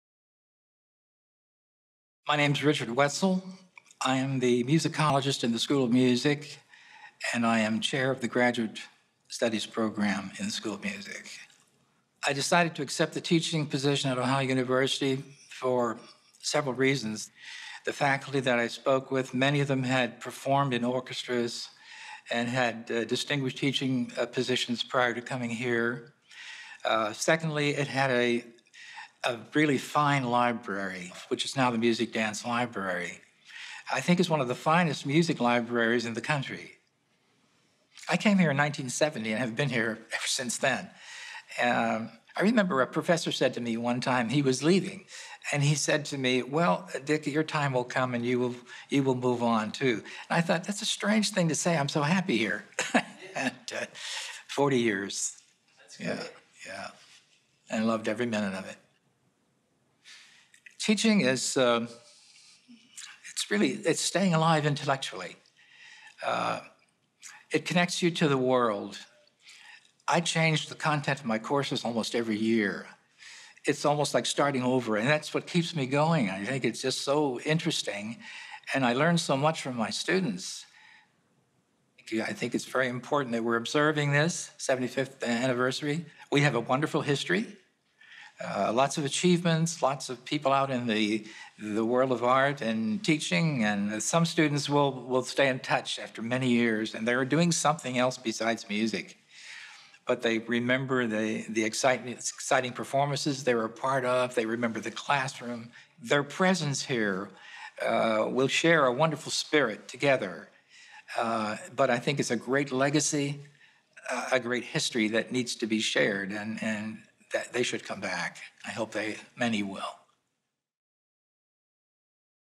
CoFA 75th Anniversary - Richard Wetzel - Music
As part of the upcoming 75th Anniversary of the College of Fine Arts we have put together 12 videos that feature Faculty and Staff and highlight the importance of art in their lives and at Ohio University.